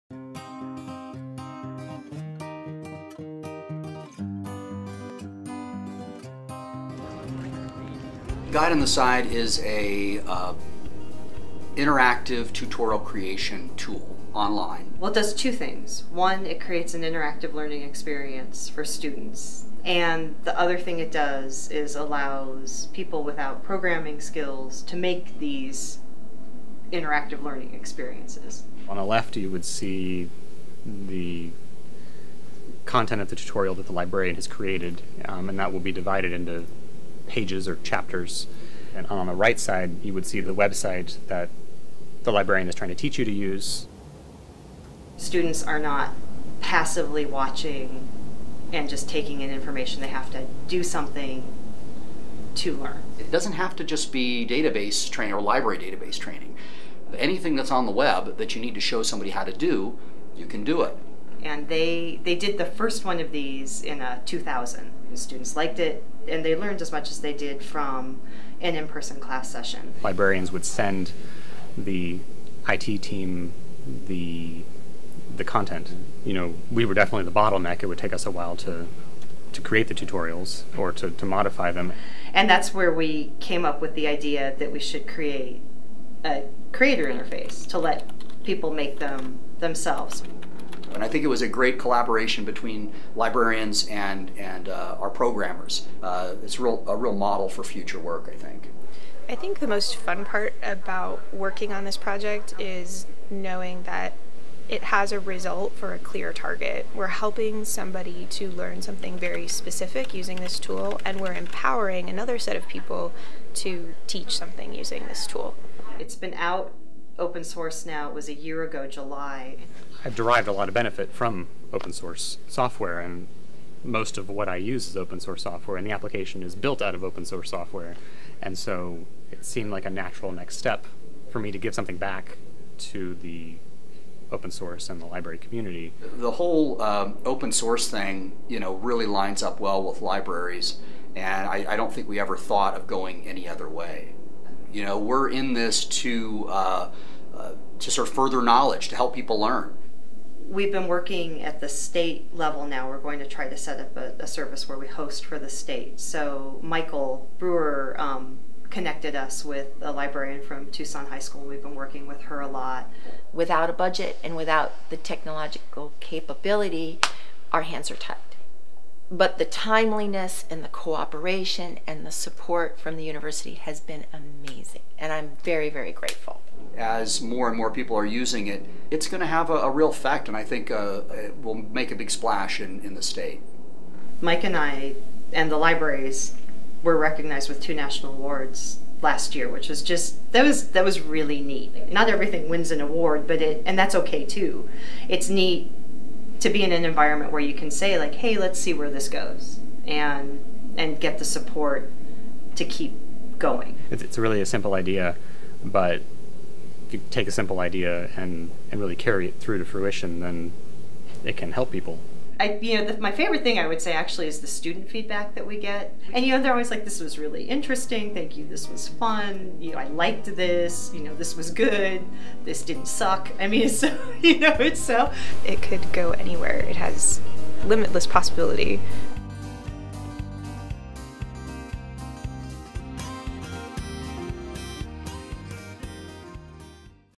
Guide on the Side
Guide on the Side is an award-winning, open-source program for creating interactive web tutorials. Developed in-house at the University of Arizona Libraries, Guide on the Side was initially designed to help students learn to navigate online databases. Since then, Guide has been adopted by institutions across the country and used as an instructional aide with numerous other types of websites.